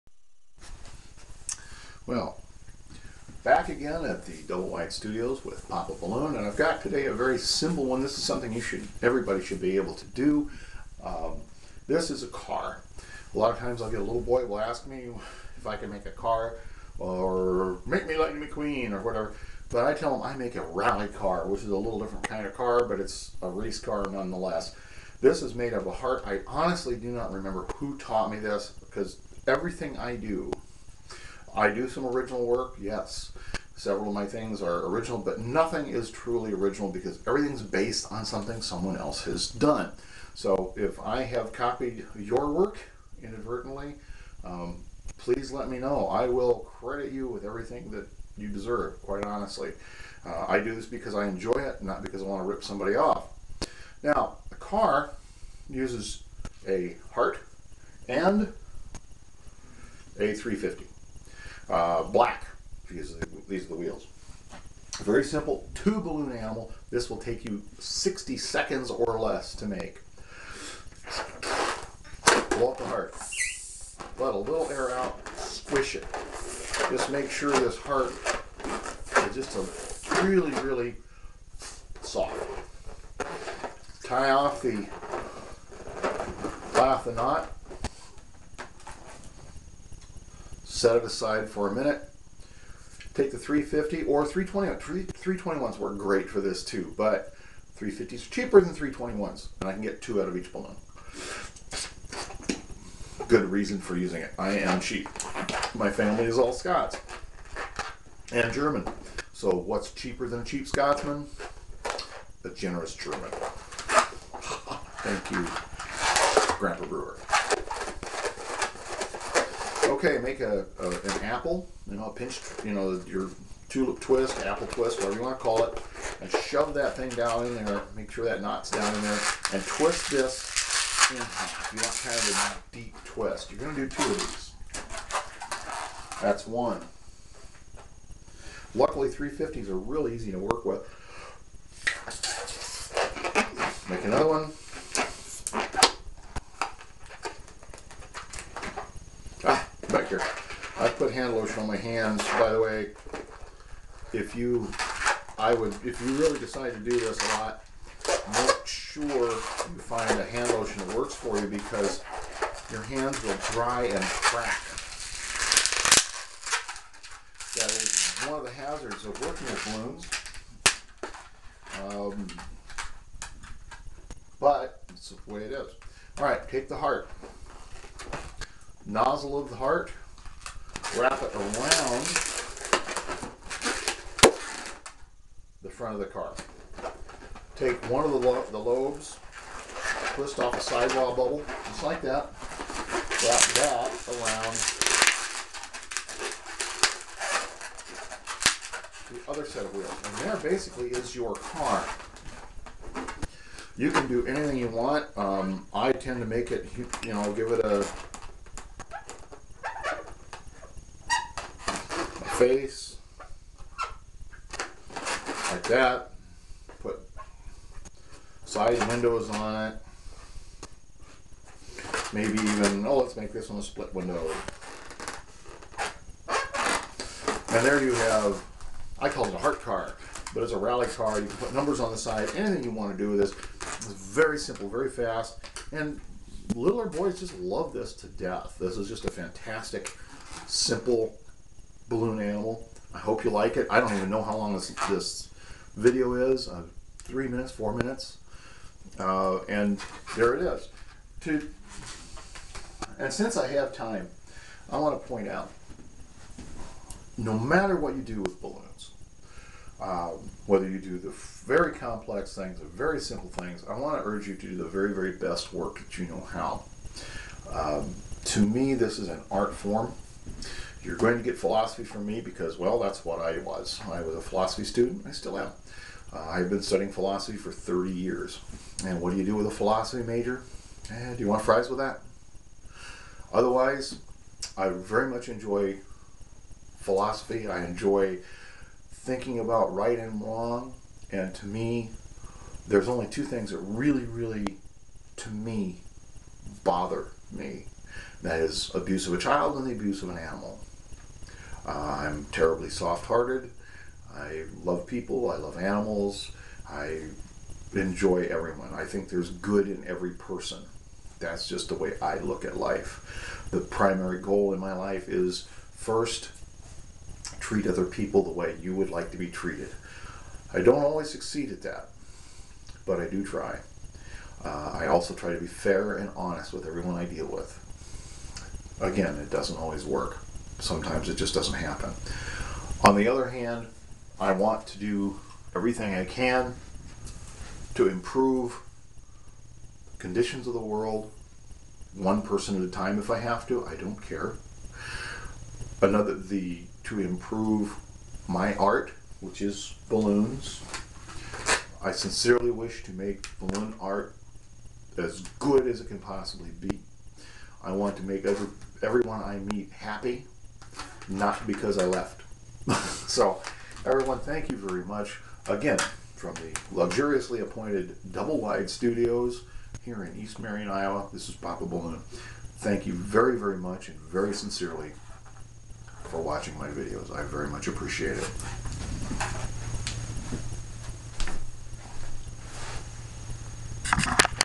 Balloon car
A simple balloon car that is great for line work and small people